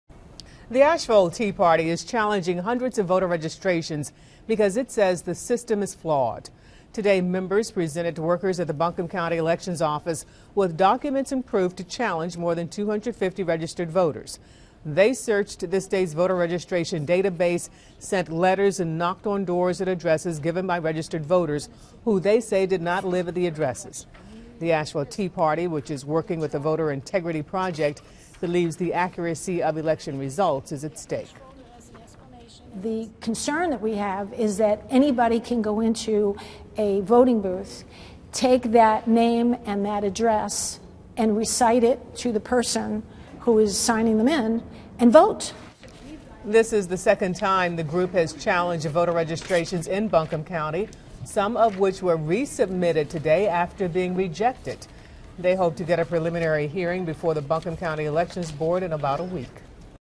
Asheville Tea Party Challenging Hundreds of Vote
ASHEVILLE, N.C. -- The Asheville Tea Party is challenging hundreds of voter registrations because it says the system is flawed.

On Thursday, members presented workers at the Buncombe County Elections Office with documents and proof to challenge more than 250 registered voters.

They searched the state's voter registration data base, sent letters and knocked on doors at addresses given by registered voters who they say did not live at the address.

The Asheville Tea Party, which is working with the Voter Integrity Project, believes the accuracy of election results is at stake.

"The concern that we have is that anybody can go into a voting booth, take that name and address and recite it to the person that is signing them in and vote," Jane Bilello with the Asheville Tea Party said.

This is the second time the group has challenged voter registrations in Buncombe County, some of which were re-submitted after being rejected.

They hope to get a preliminary hearing before the Buncombe County Elections Board in about a week.